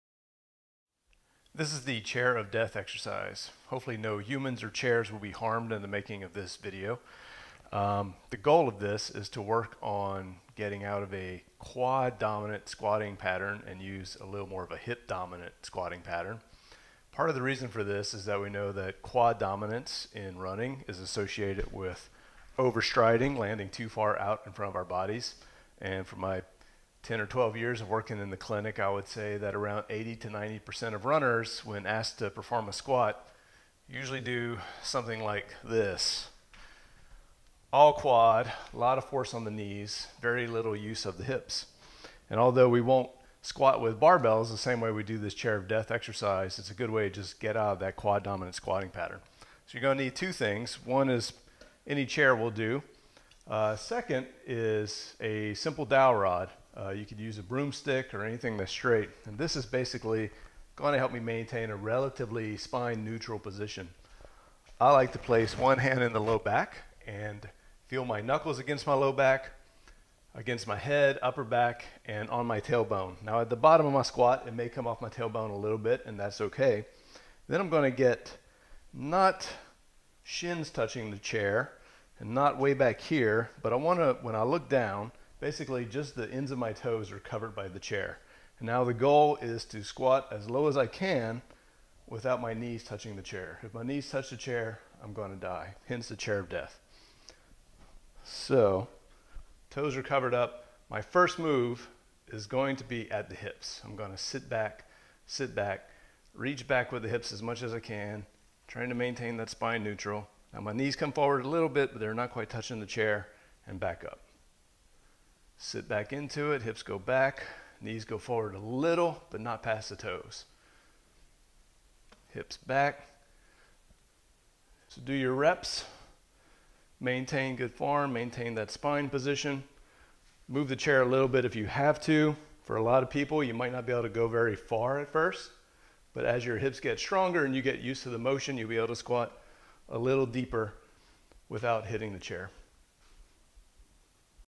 “Chair of Death” Squats
We use this exercise to move away from a quad dominant squat pattern to a more glute dominant pattern, and eventually to a more even spread of using glutes and quads. This can be very helpful for runners with patellofemoral pain limiting their ability to squat.